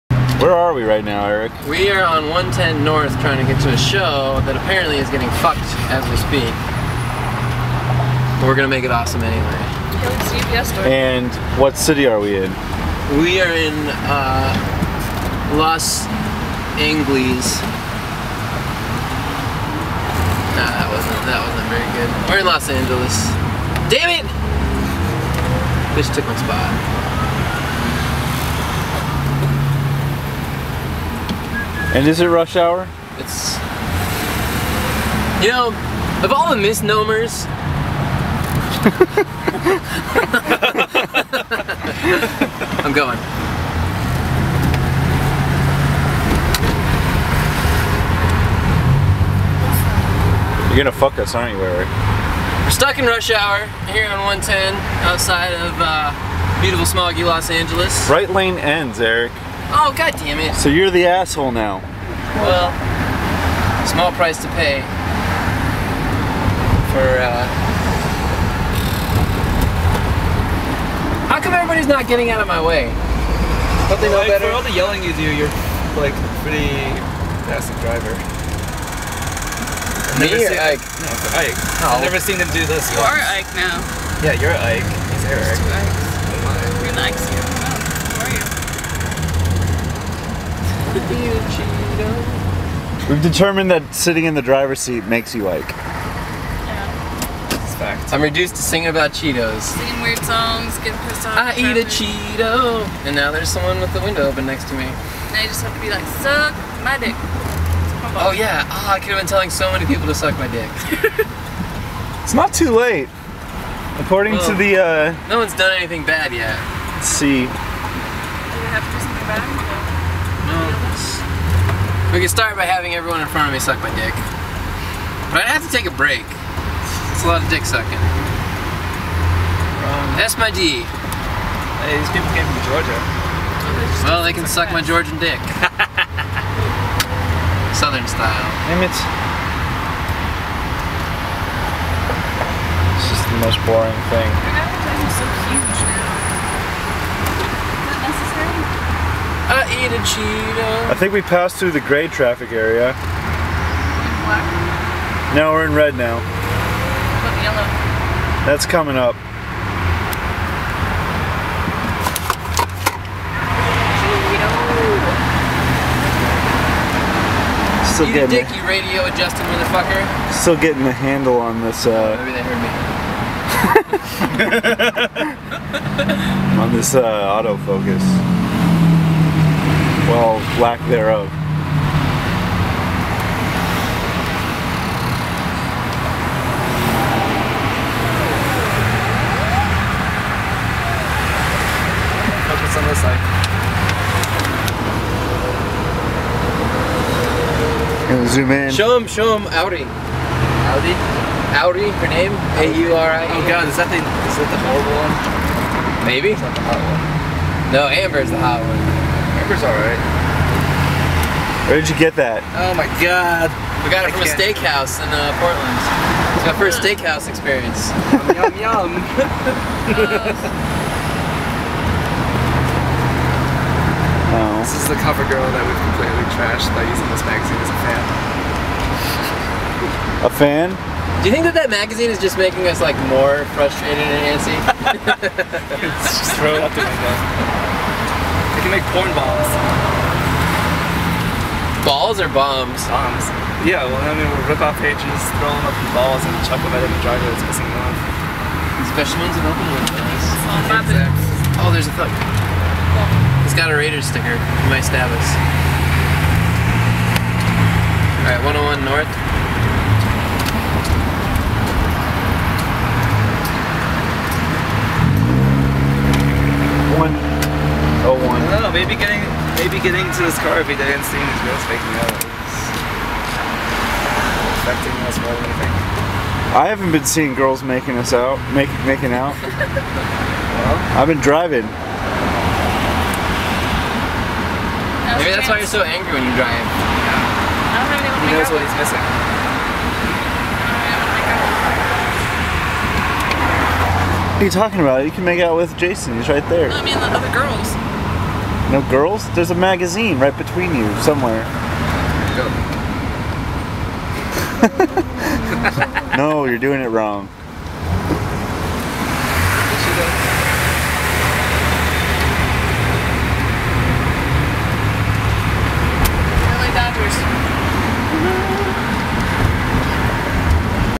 Eric Tackles LA Rush Hour
This is how Eric deals with LA rush hour while on tour with Swingset Showdown. 2013